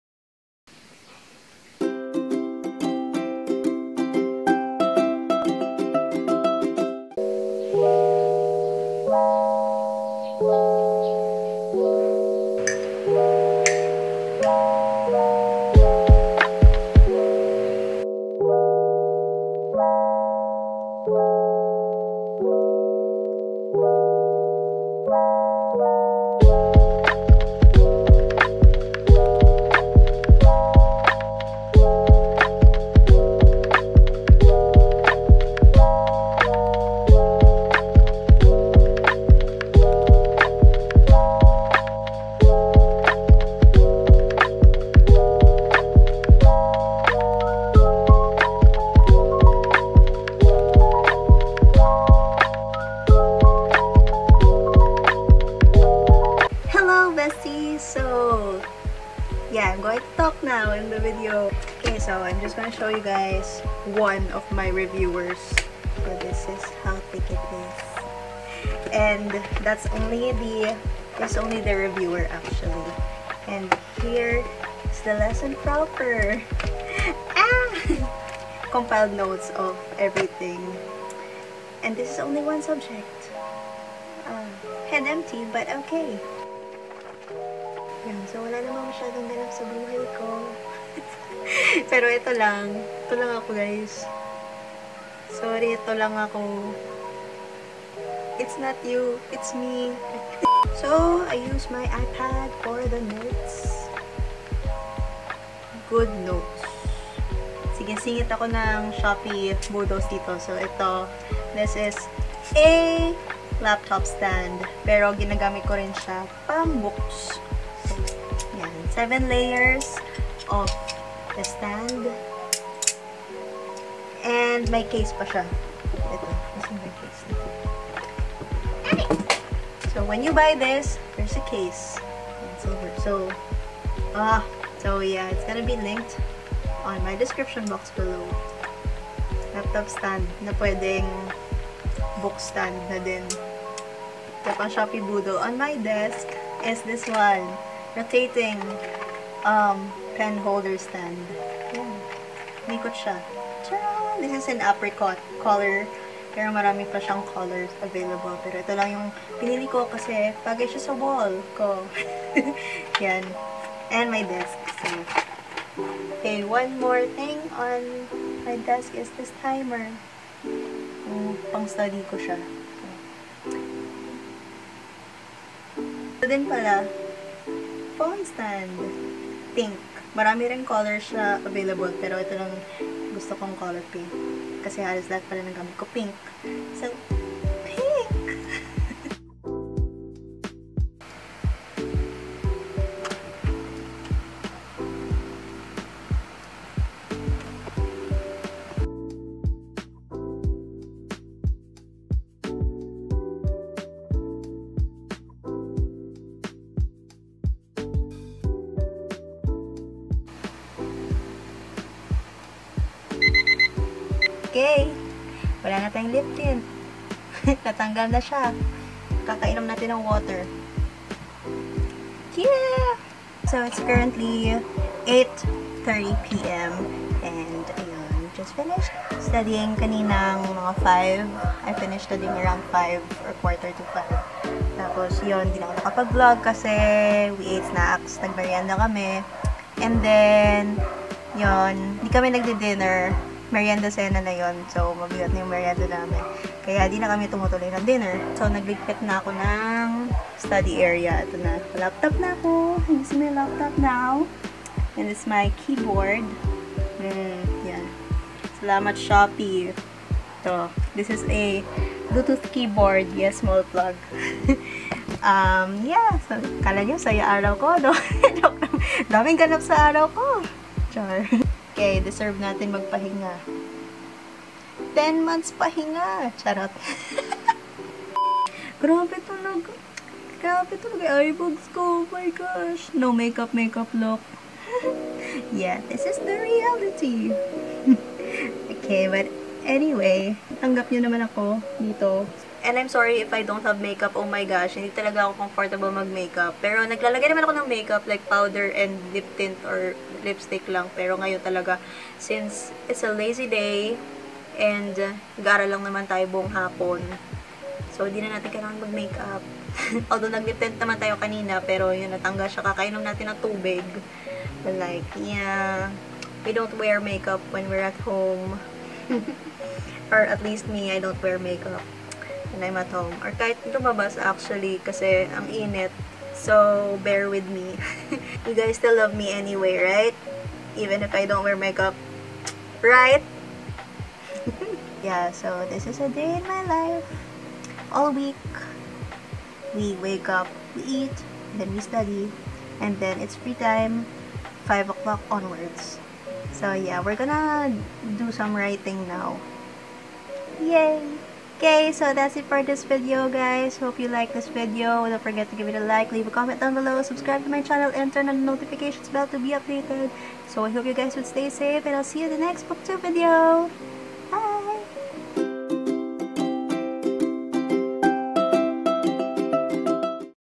A Day In My Life | booksbytheshelves
hello book besties! hooray for being back in this channel?? LOL howru? how's it going???

Well so I asked if *you* wanted a day in my life (not that there's anything interesting in my life LOL) so here it is. Routine is just wake up-eat-study-eat??? HAHAHA that is how it is here.

Shopee links!

stay safe, take care! xx